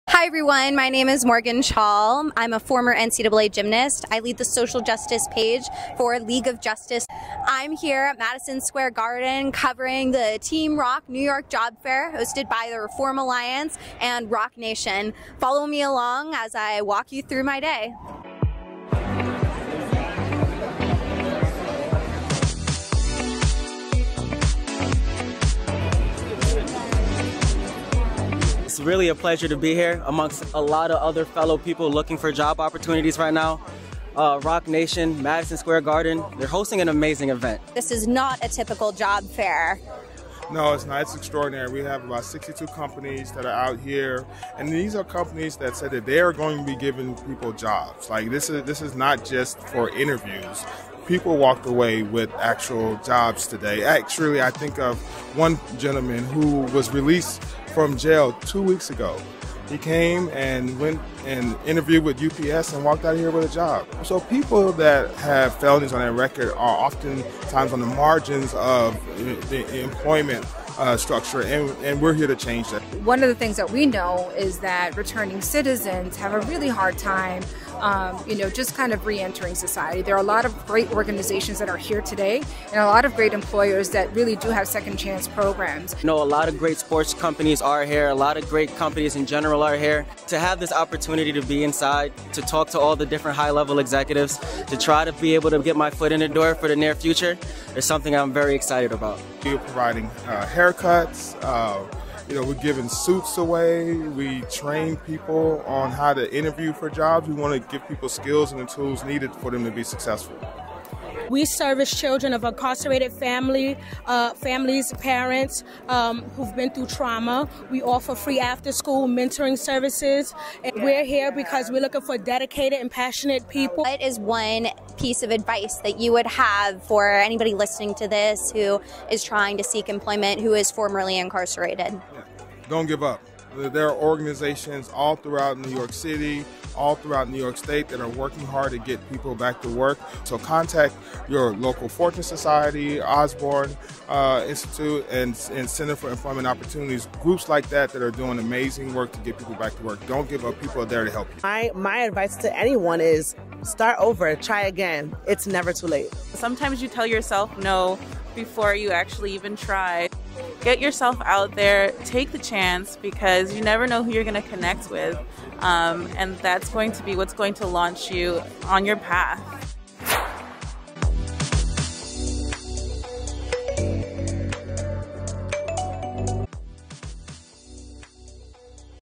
League of Justice Covers The Reform Alliance & Roc Nation NYC Job Fair
Jay Z's Roc Nation & Criminal Justice Non-Profit Reform Alliance Create a New Standard for Job Fairs. People Actually Leave with Jobs & new Skills, Even Makeovers! Watch as LOJ Superspondent Morgan Chall Checks it Out!